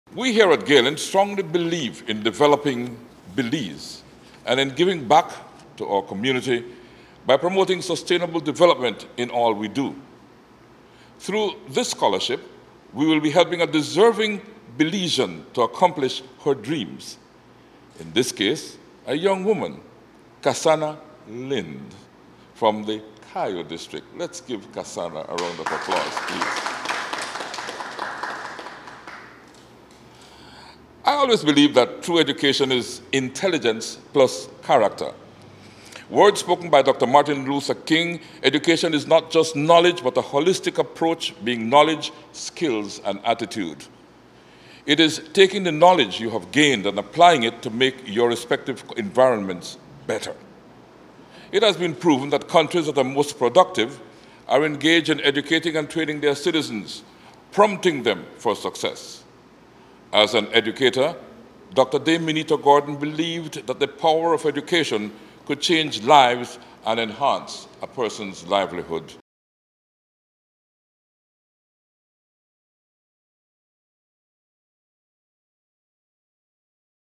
Galen University Launches the Doctor Dame Minita Gordon Scholarship Program | Pt 1
The annual Doctor Dame Minita Gordon Scholarship was launched at Galen University on Thursday. It is named after Belize's first post-Independence Head of State, who died on January 1, 2021 at age 90. Chairman of the university's Board of Trustees Doctor Rene Villanueva Senior spoke at the launch.